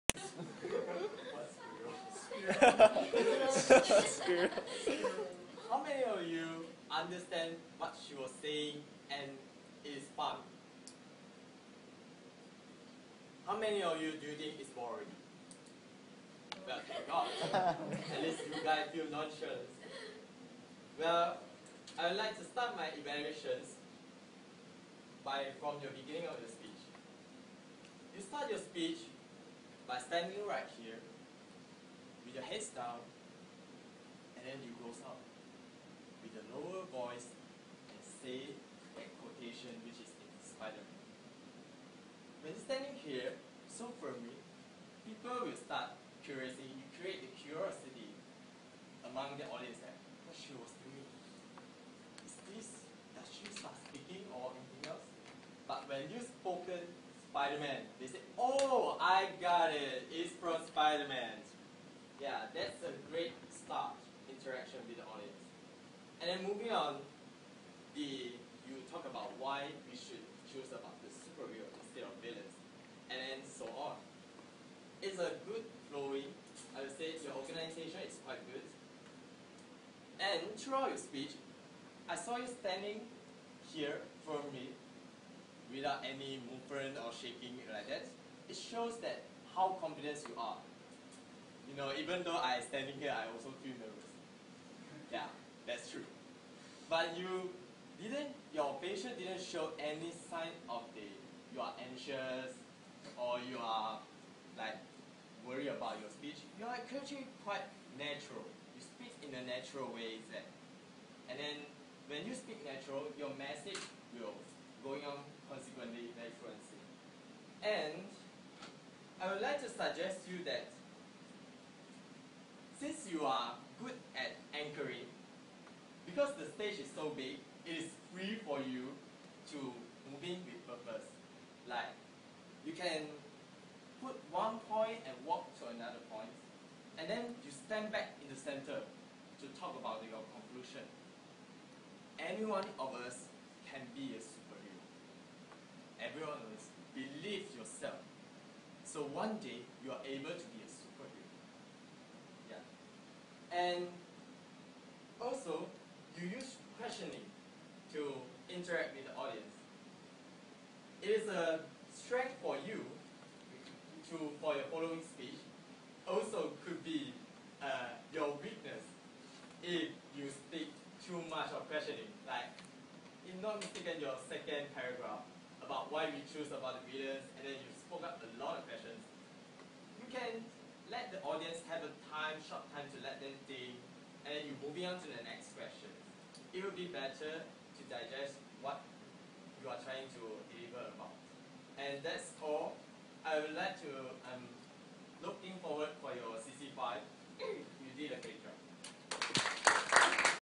Evaluation Speech 1
Spoken By Teoh Kay Ment, CC
Evaluation to Denise, TM for her CC#4

HELP University Toastmasters Club (18/1/2014)